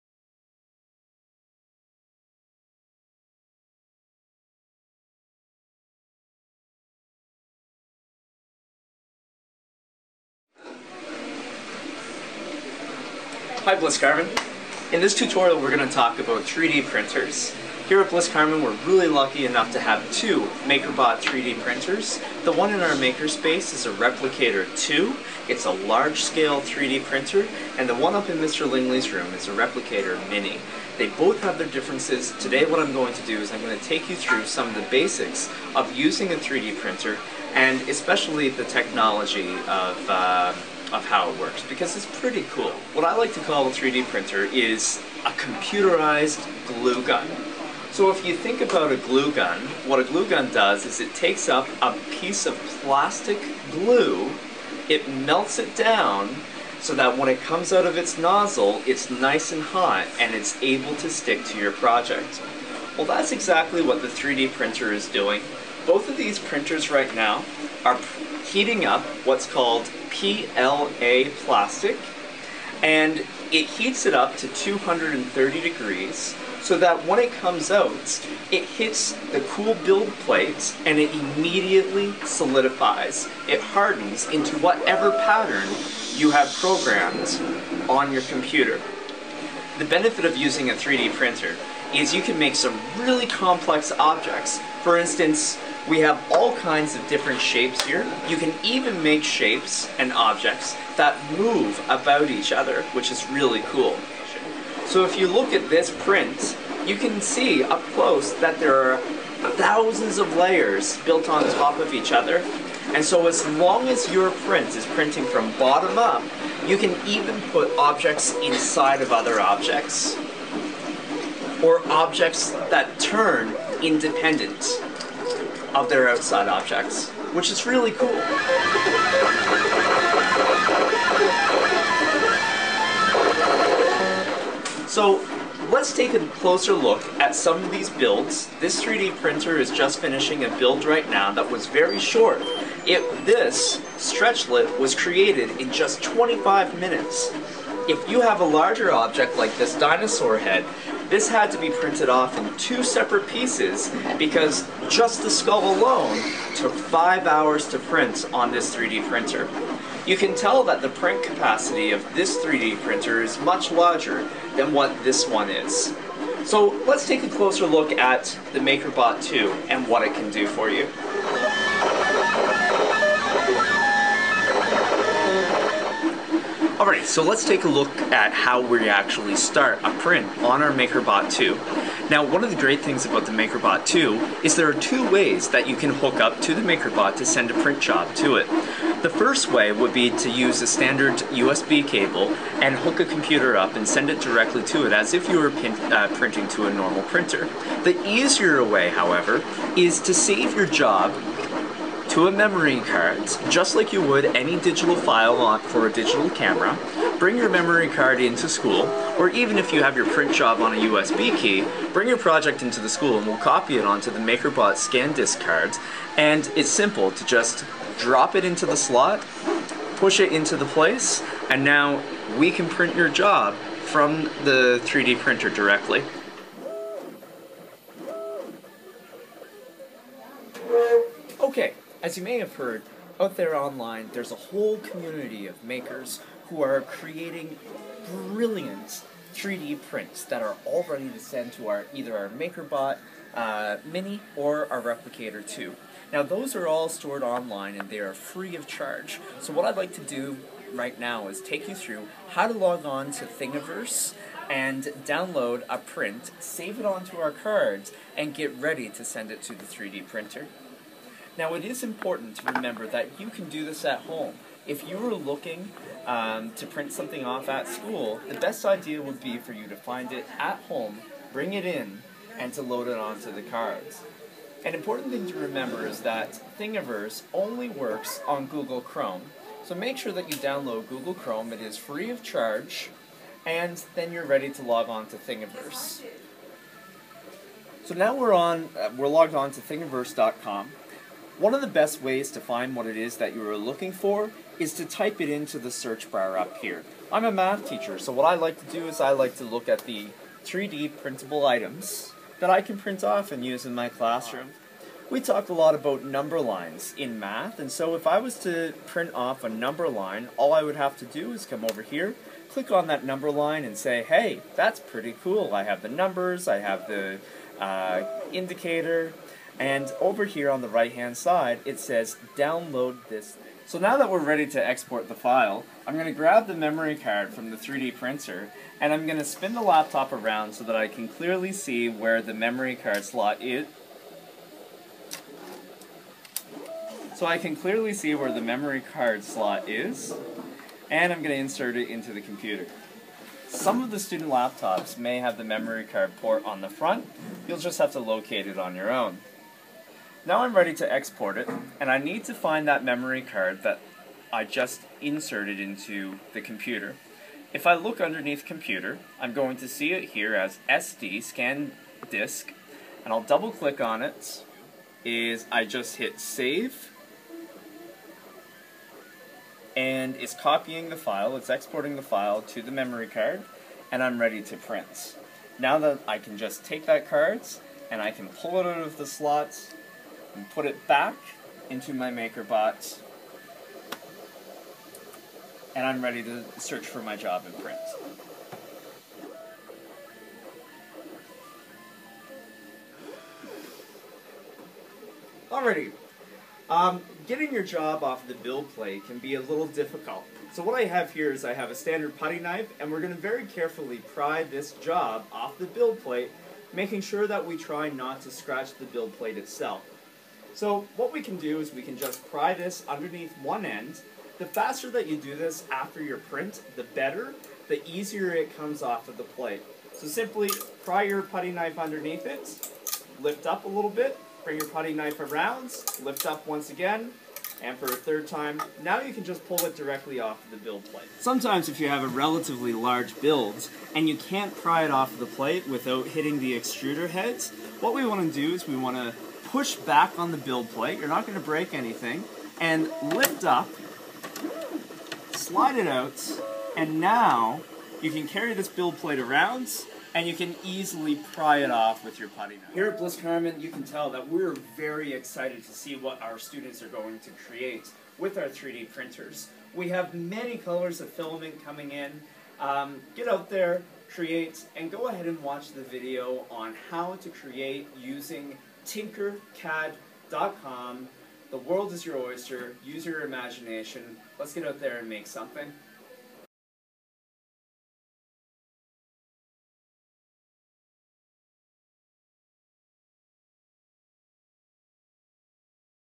3D Printer Tutorial
*EDIT: This version does not have the two minutes of black video in the middle of the tutorial.*

Learn how to operate our Makerbot Replicator 2 and how to set up a basic print design using tinkercad.com.